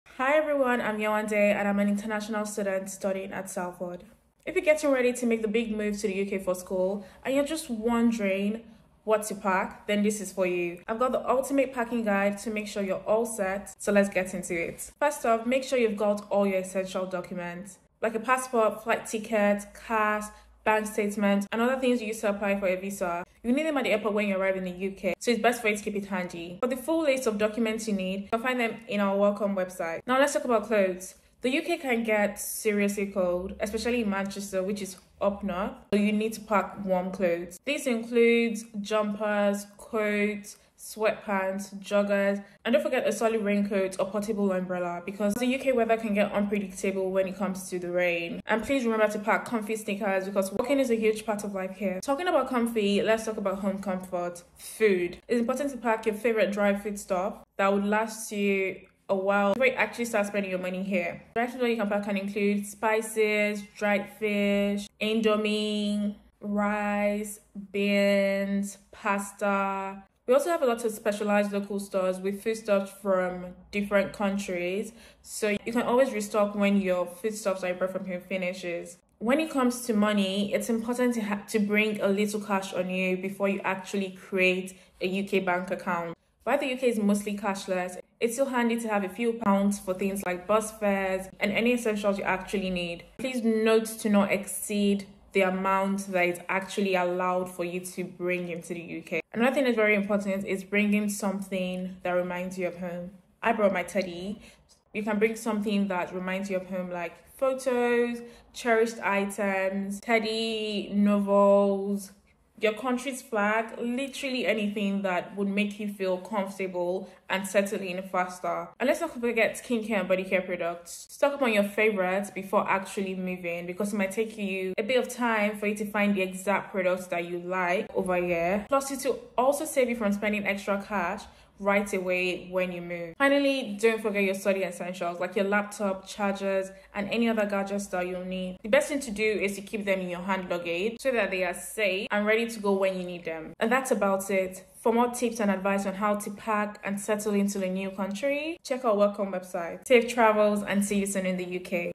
What to Pack as an International Student
Are you an international student coming to the UK to study? Then you might be wondering what are the essential things you need to put in your suitcase.

In this video, current international student Yewande, explains what she would recommend. Take a look, and then remember to look at the further information on the websites below: